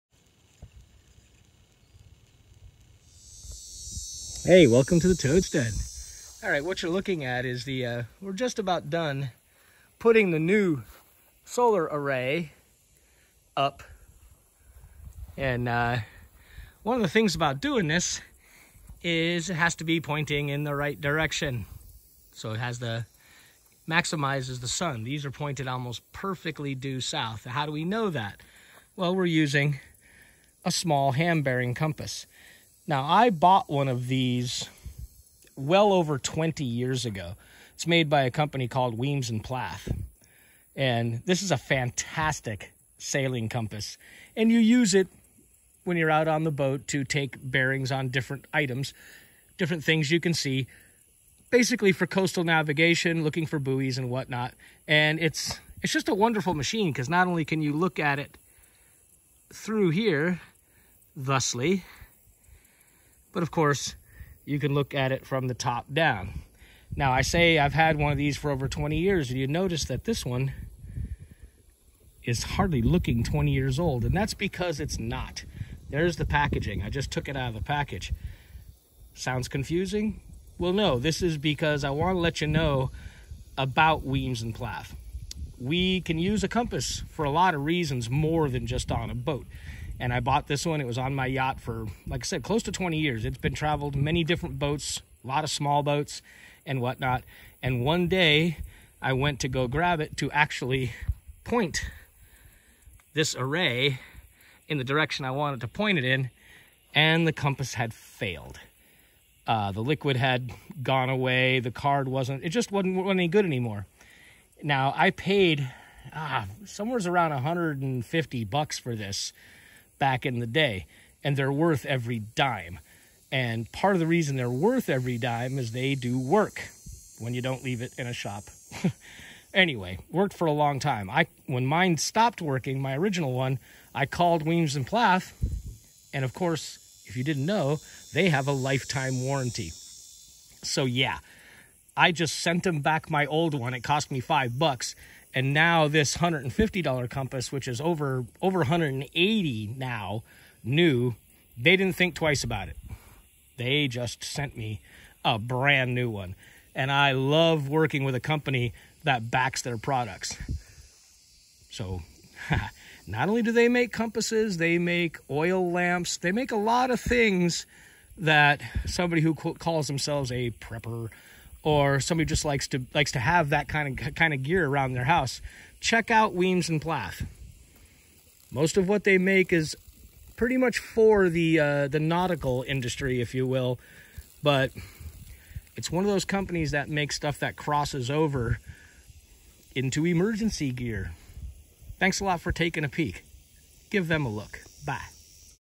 Company Kudos. Weems and Plath
Sometimes here on the Toadstead, we like to shout-out companies who do the right thing. Weems and Plath is one such company. While they are well known in the boating world, not many “preppers” May be familiar with their gear. Primarily makers of mid-high end nautical equipment, Weems and Plath DO make many products that can (and in my opinion SHOULD) be on the short list for folks looking for lighting, navigation (as the compass seen here) and much more.